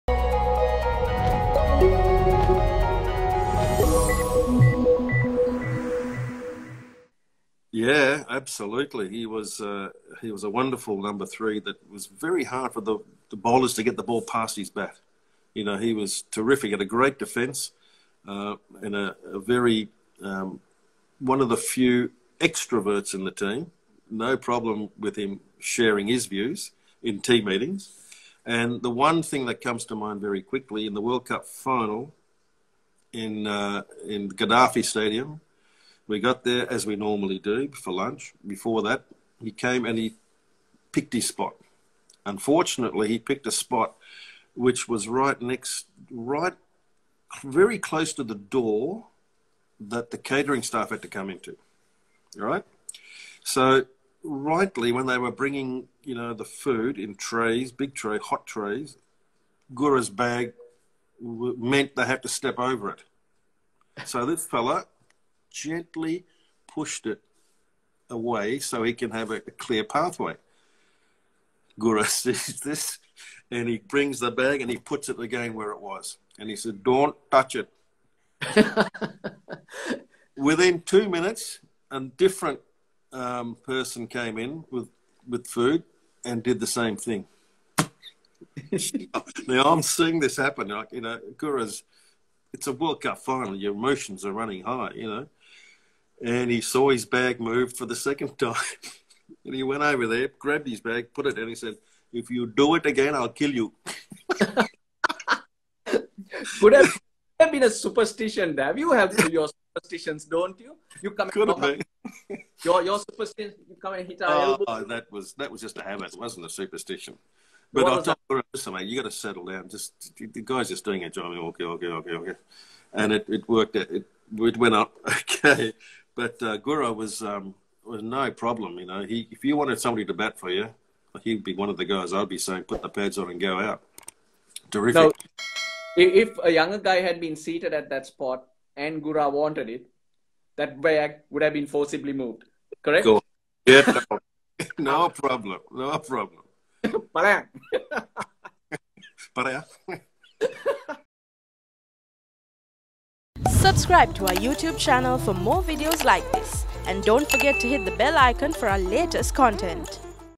Whatmore reveals funny story about Asanka Gurusinha
Former Sri Lankan head coach Dav Whatmore revealed a funny incident that happened during the 1996 World Cup finals.

He shared this story participating in an Instagram Live chat hosted by renown commentator Russel Arnold.